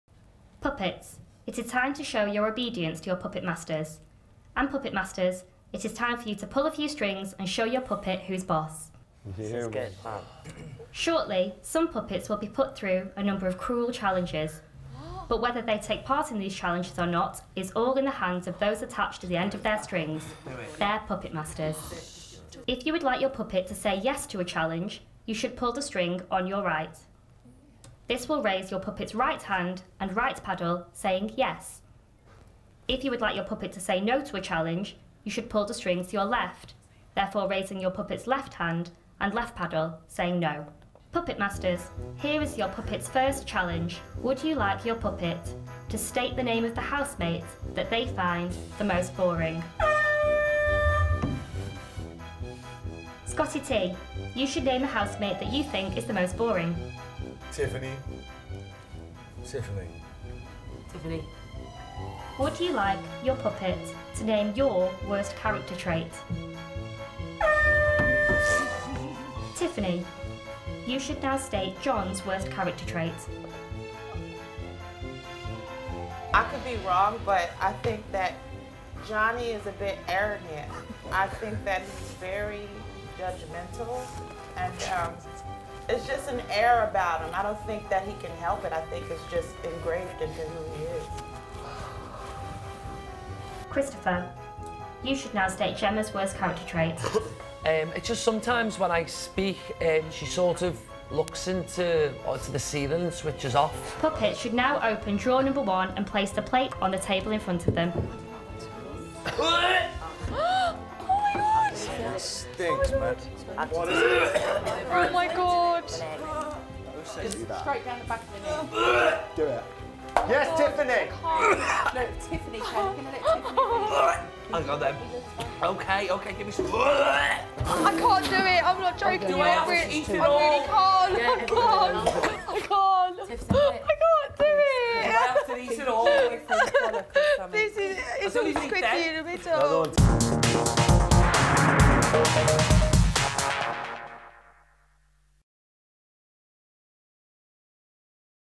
Task - Pull My Strings | Day 8
As part of today’s shopping task, the puppet masters have the power to put their puppets through some pretty extreme challenges...